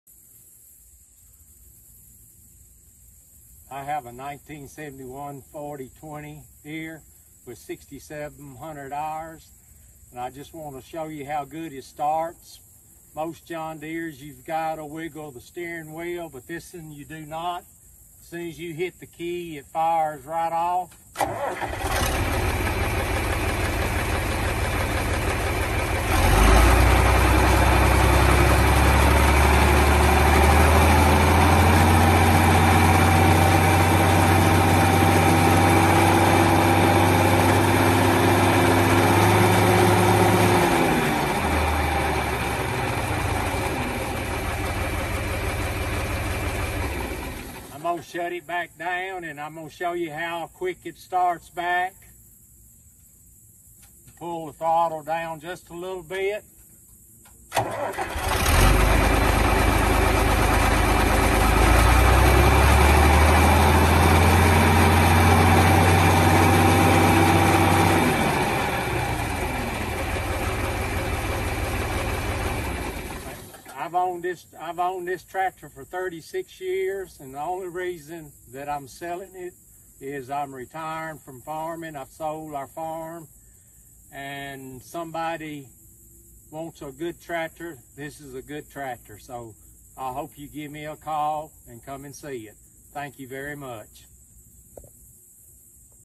1971 JOHN DEERE 4020 For Sale
1971 John Deere 4020 with synchronized transmission. It's a 2wd with many new parts. The tractor has new back rims (back tires 85% left), new front tires, new side screens, new decals, new full paint job, new fenders, new seat, and a new steering wheel. This tractor has a rebuilt clutch with less than 20 hrs on it. The tractor has primarily been used for bailing hay and bush hogging. It still runs great and starts easily (see video).